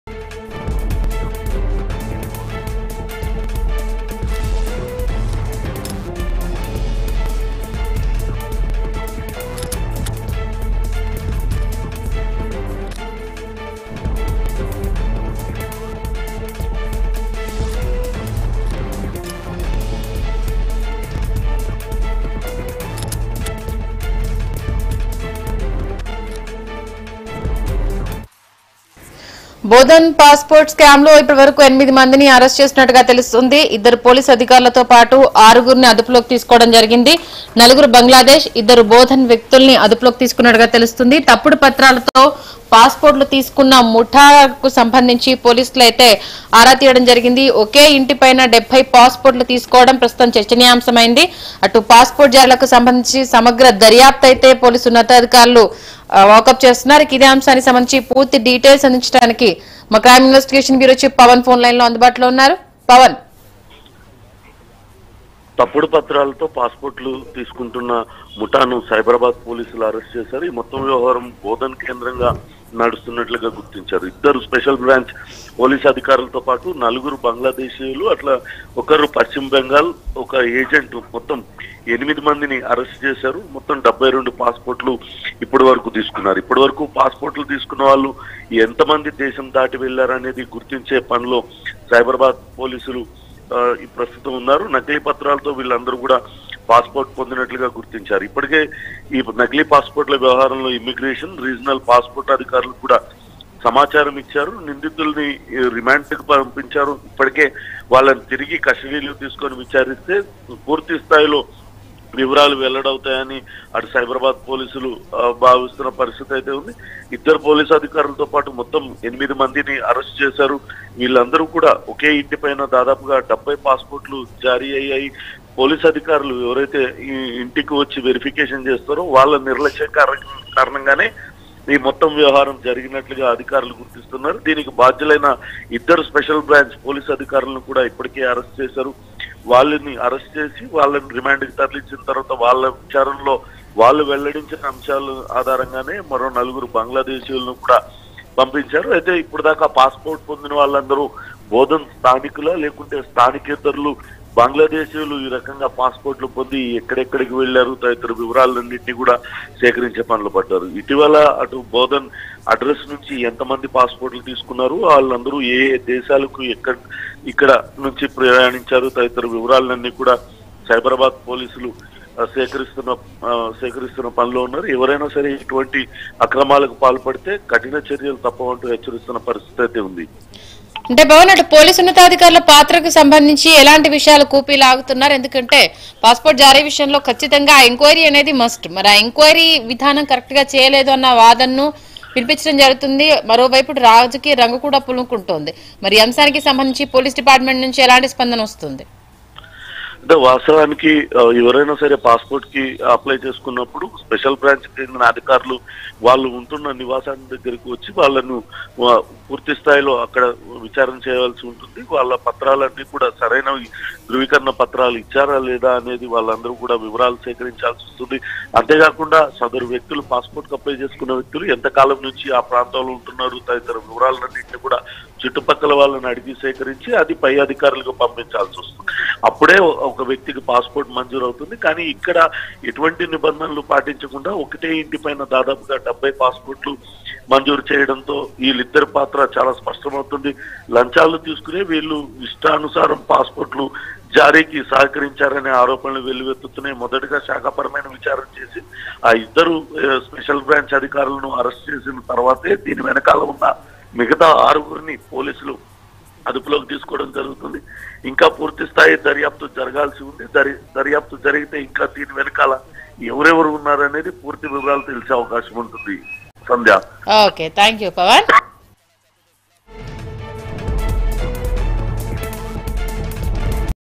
ఓకే అడ్రస్‌తో ఏకంగా 70 పాస్‌పోర్టులు...8 మంది అరెస్ట్ | Bodhan Passport Scam | CVR News
ఓకే అడ్రస్‌తో 70 పాస్‌పోర్టులు...8 మంది అరెస్ట్ | Bodhan Passport Scam | CVR News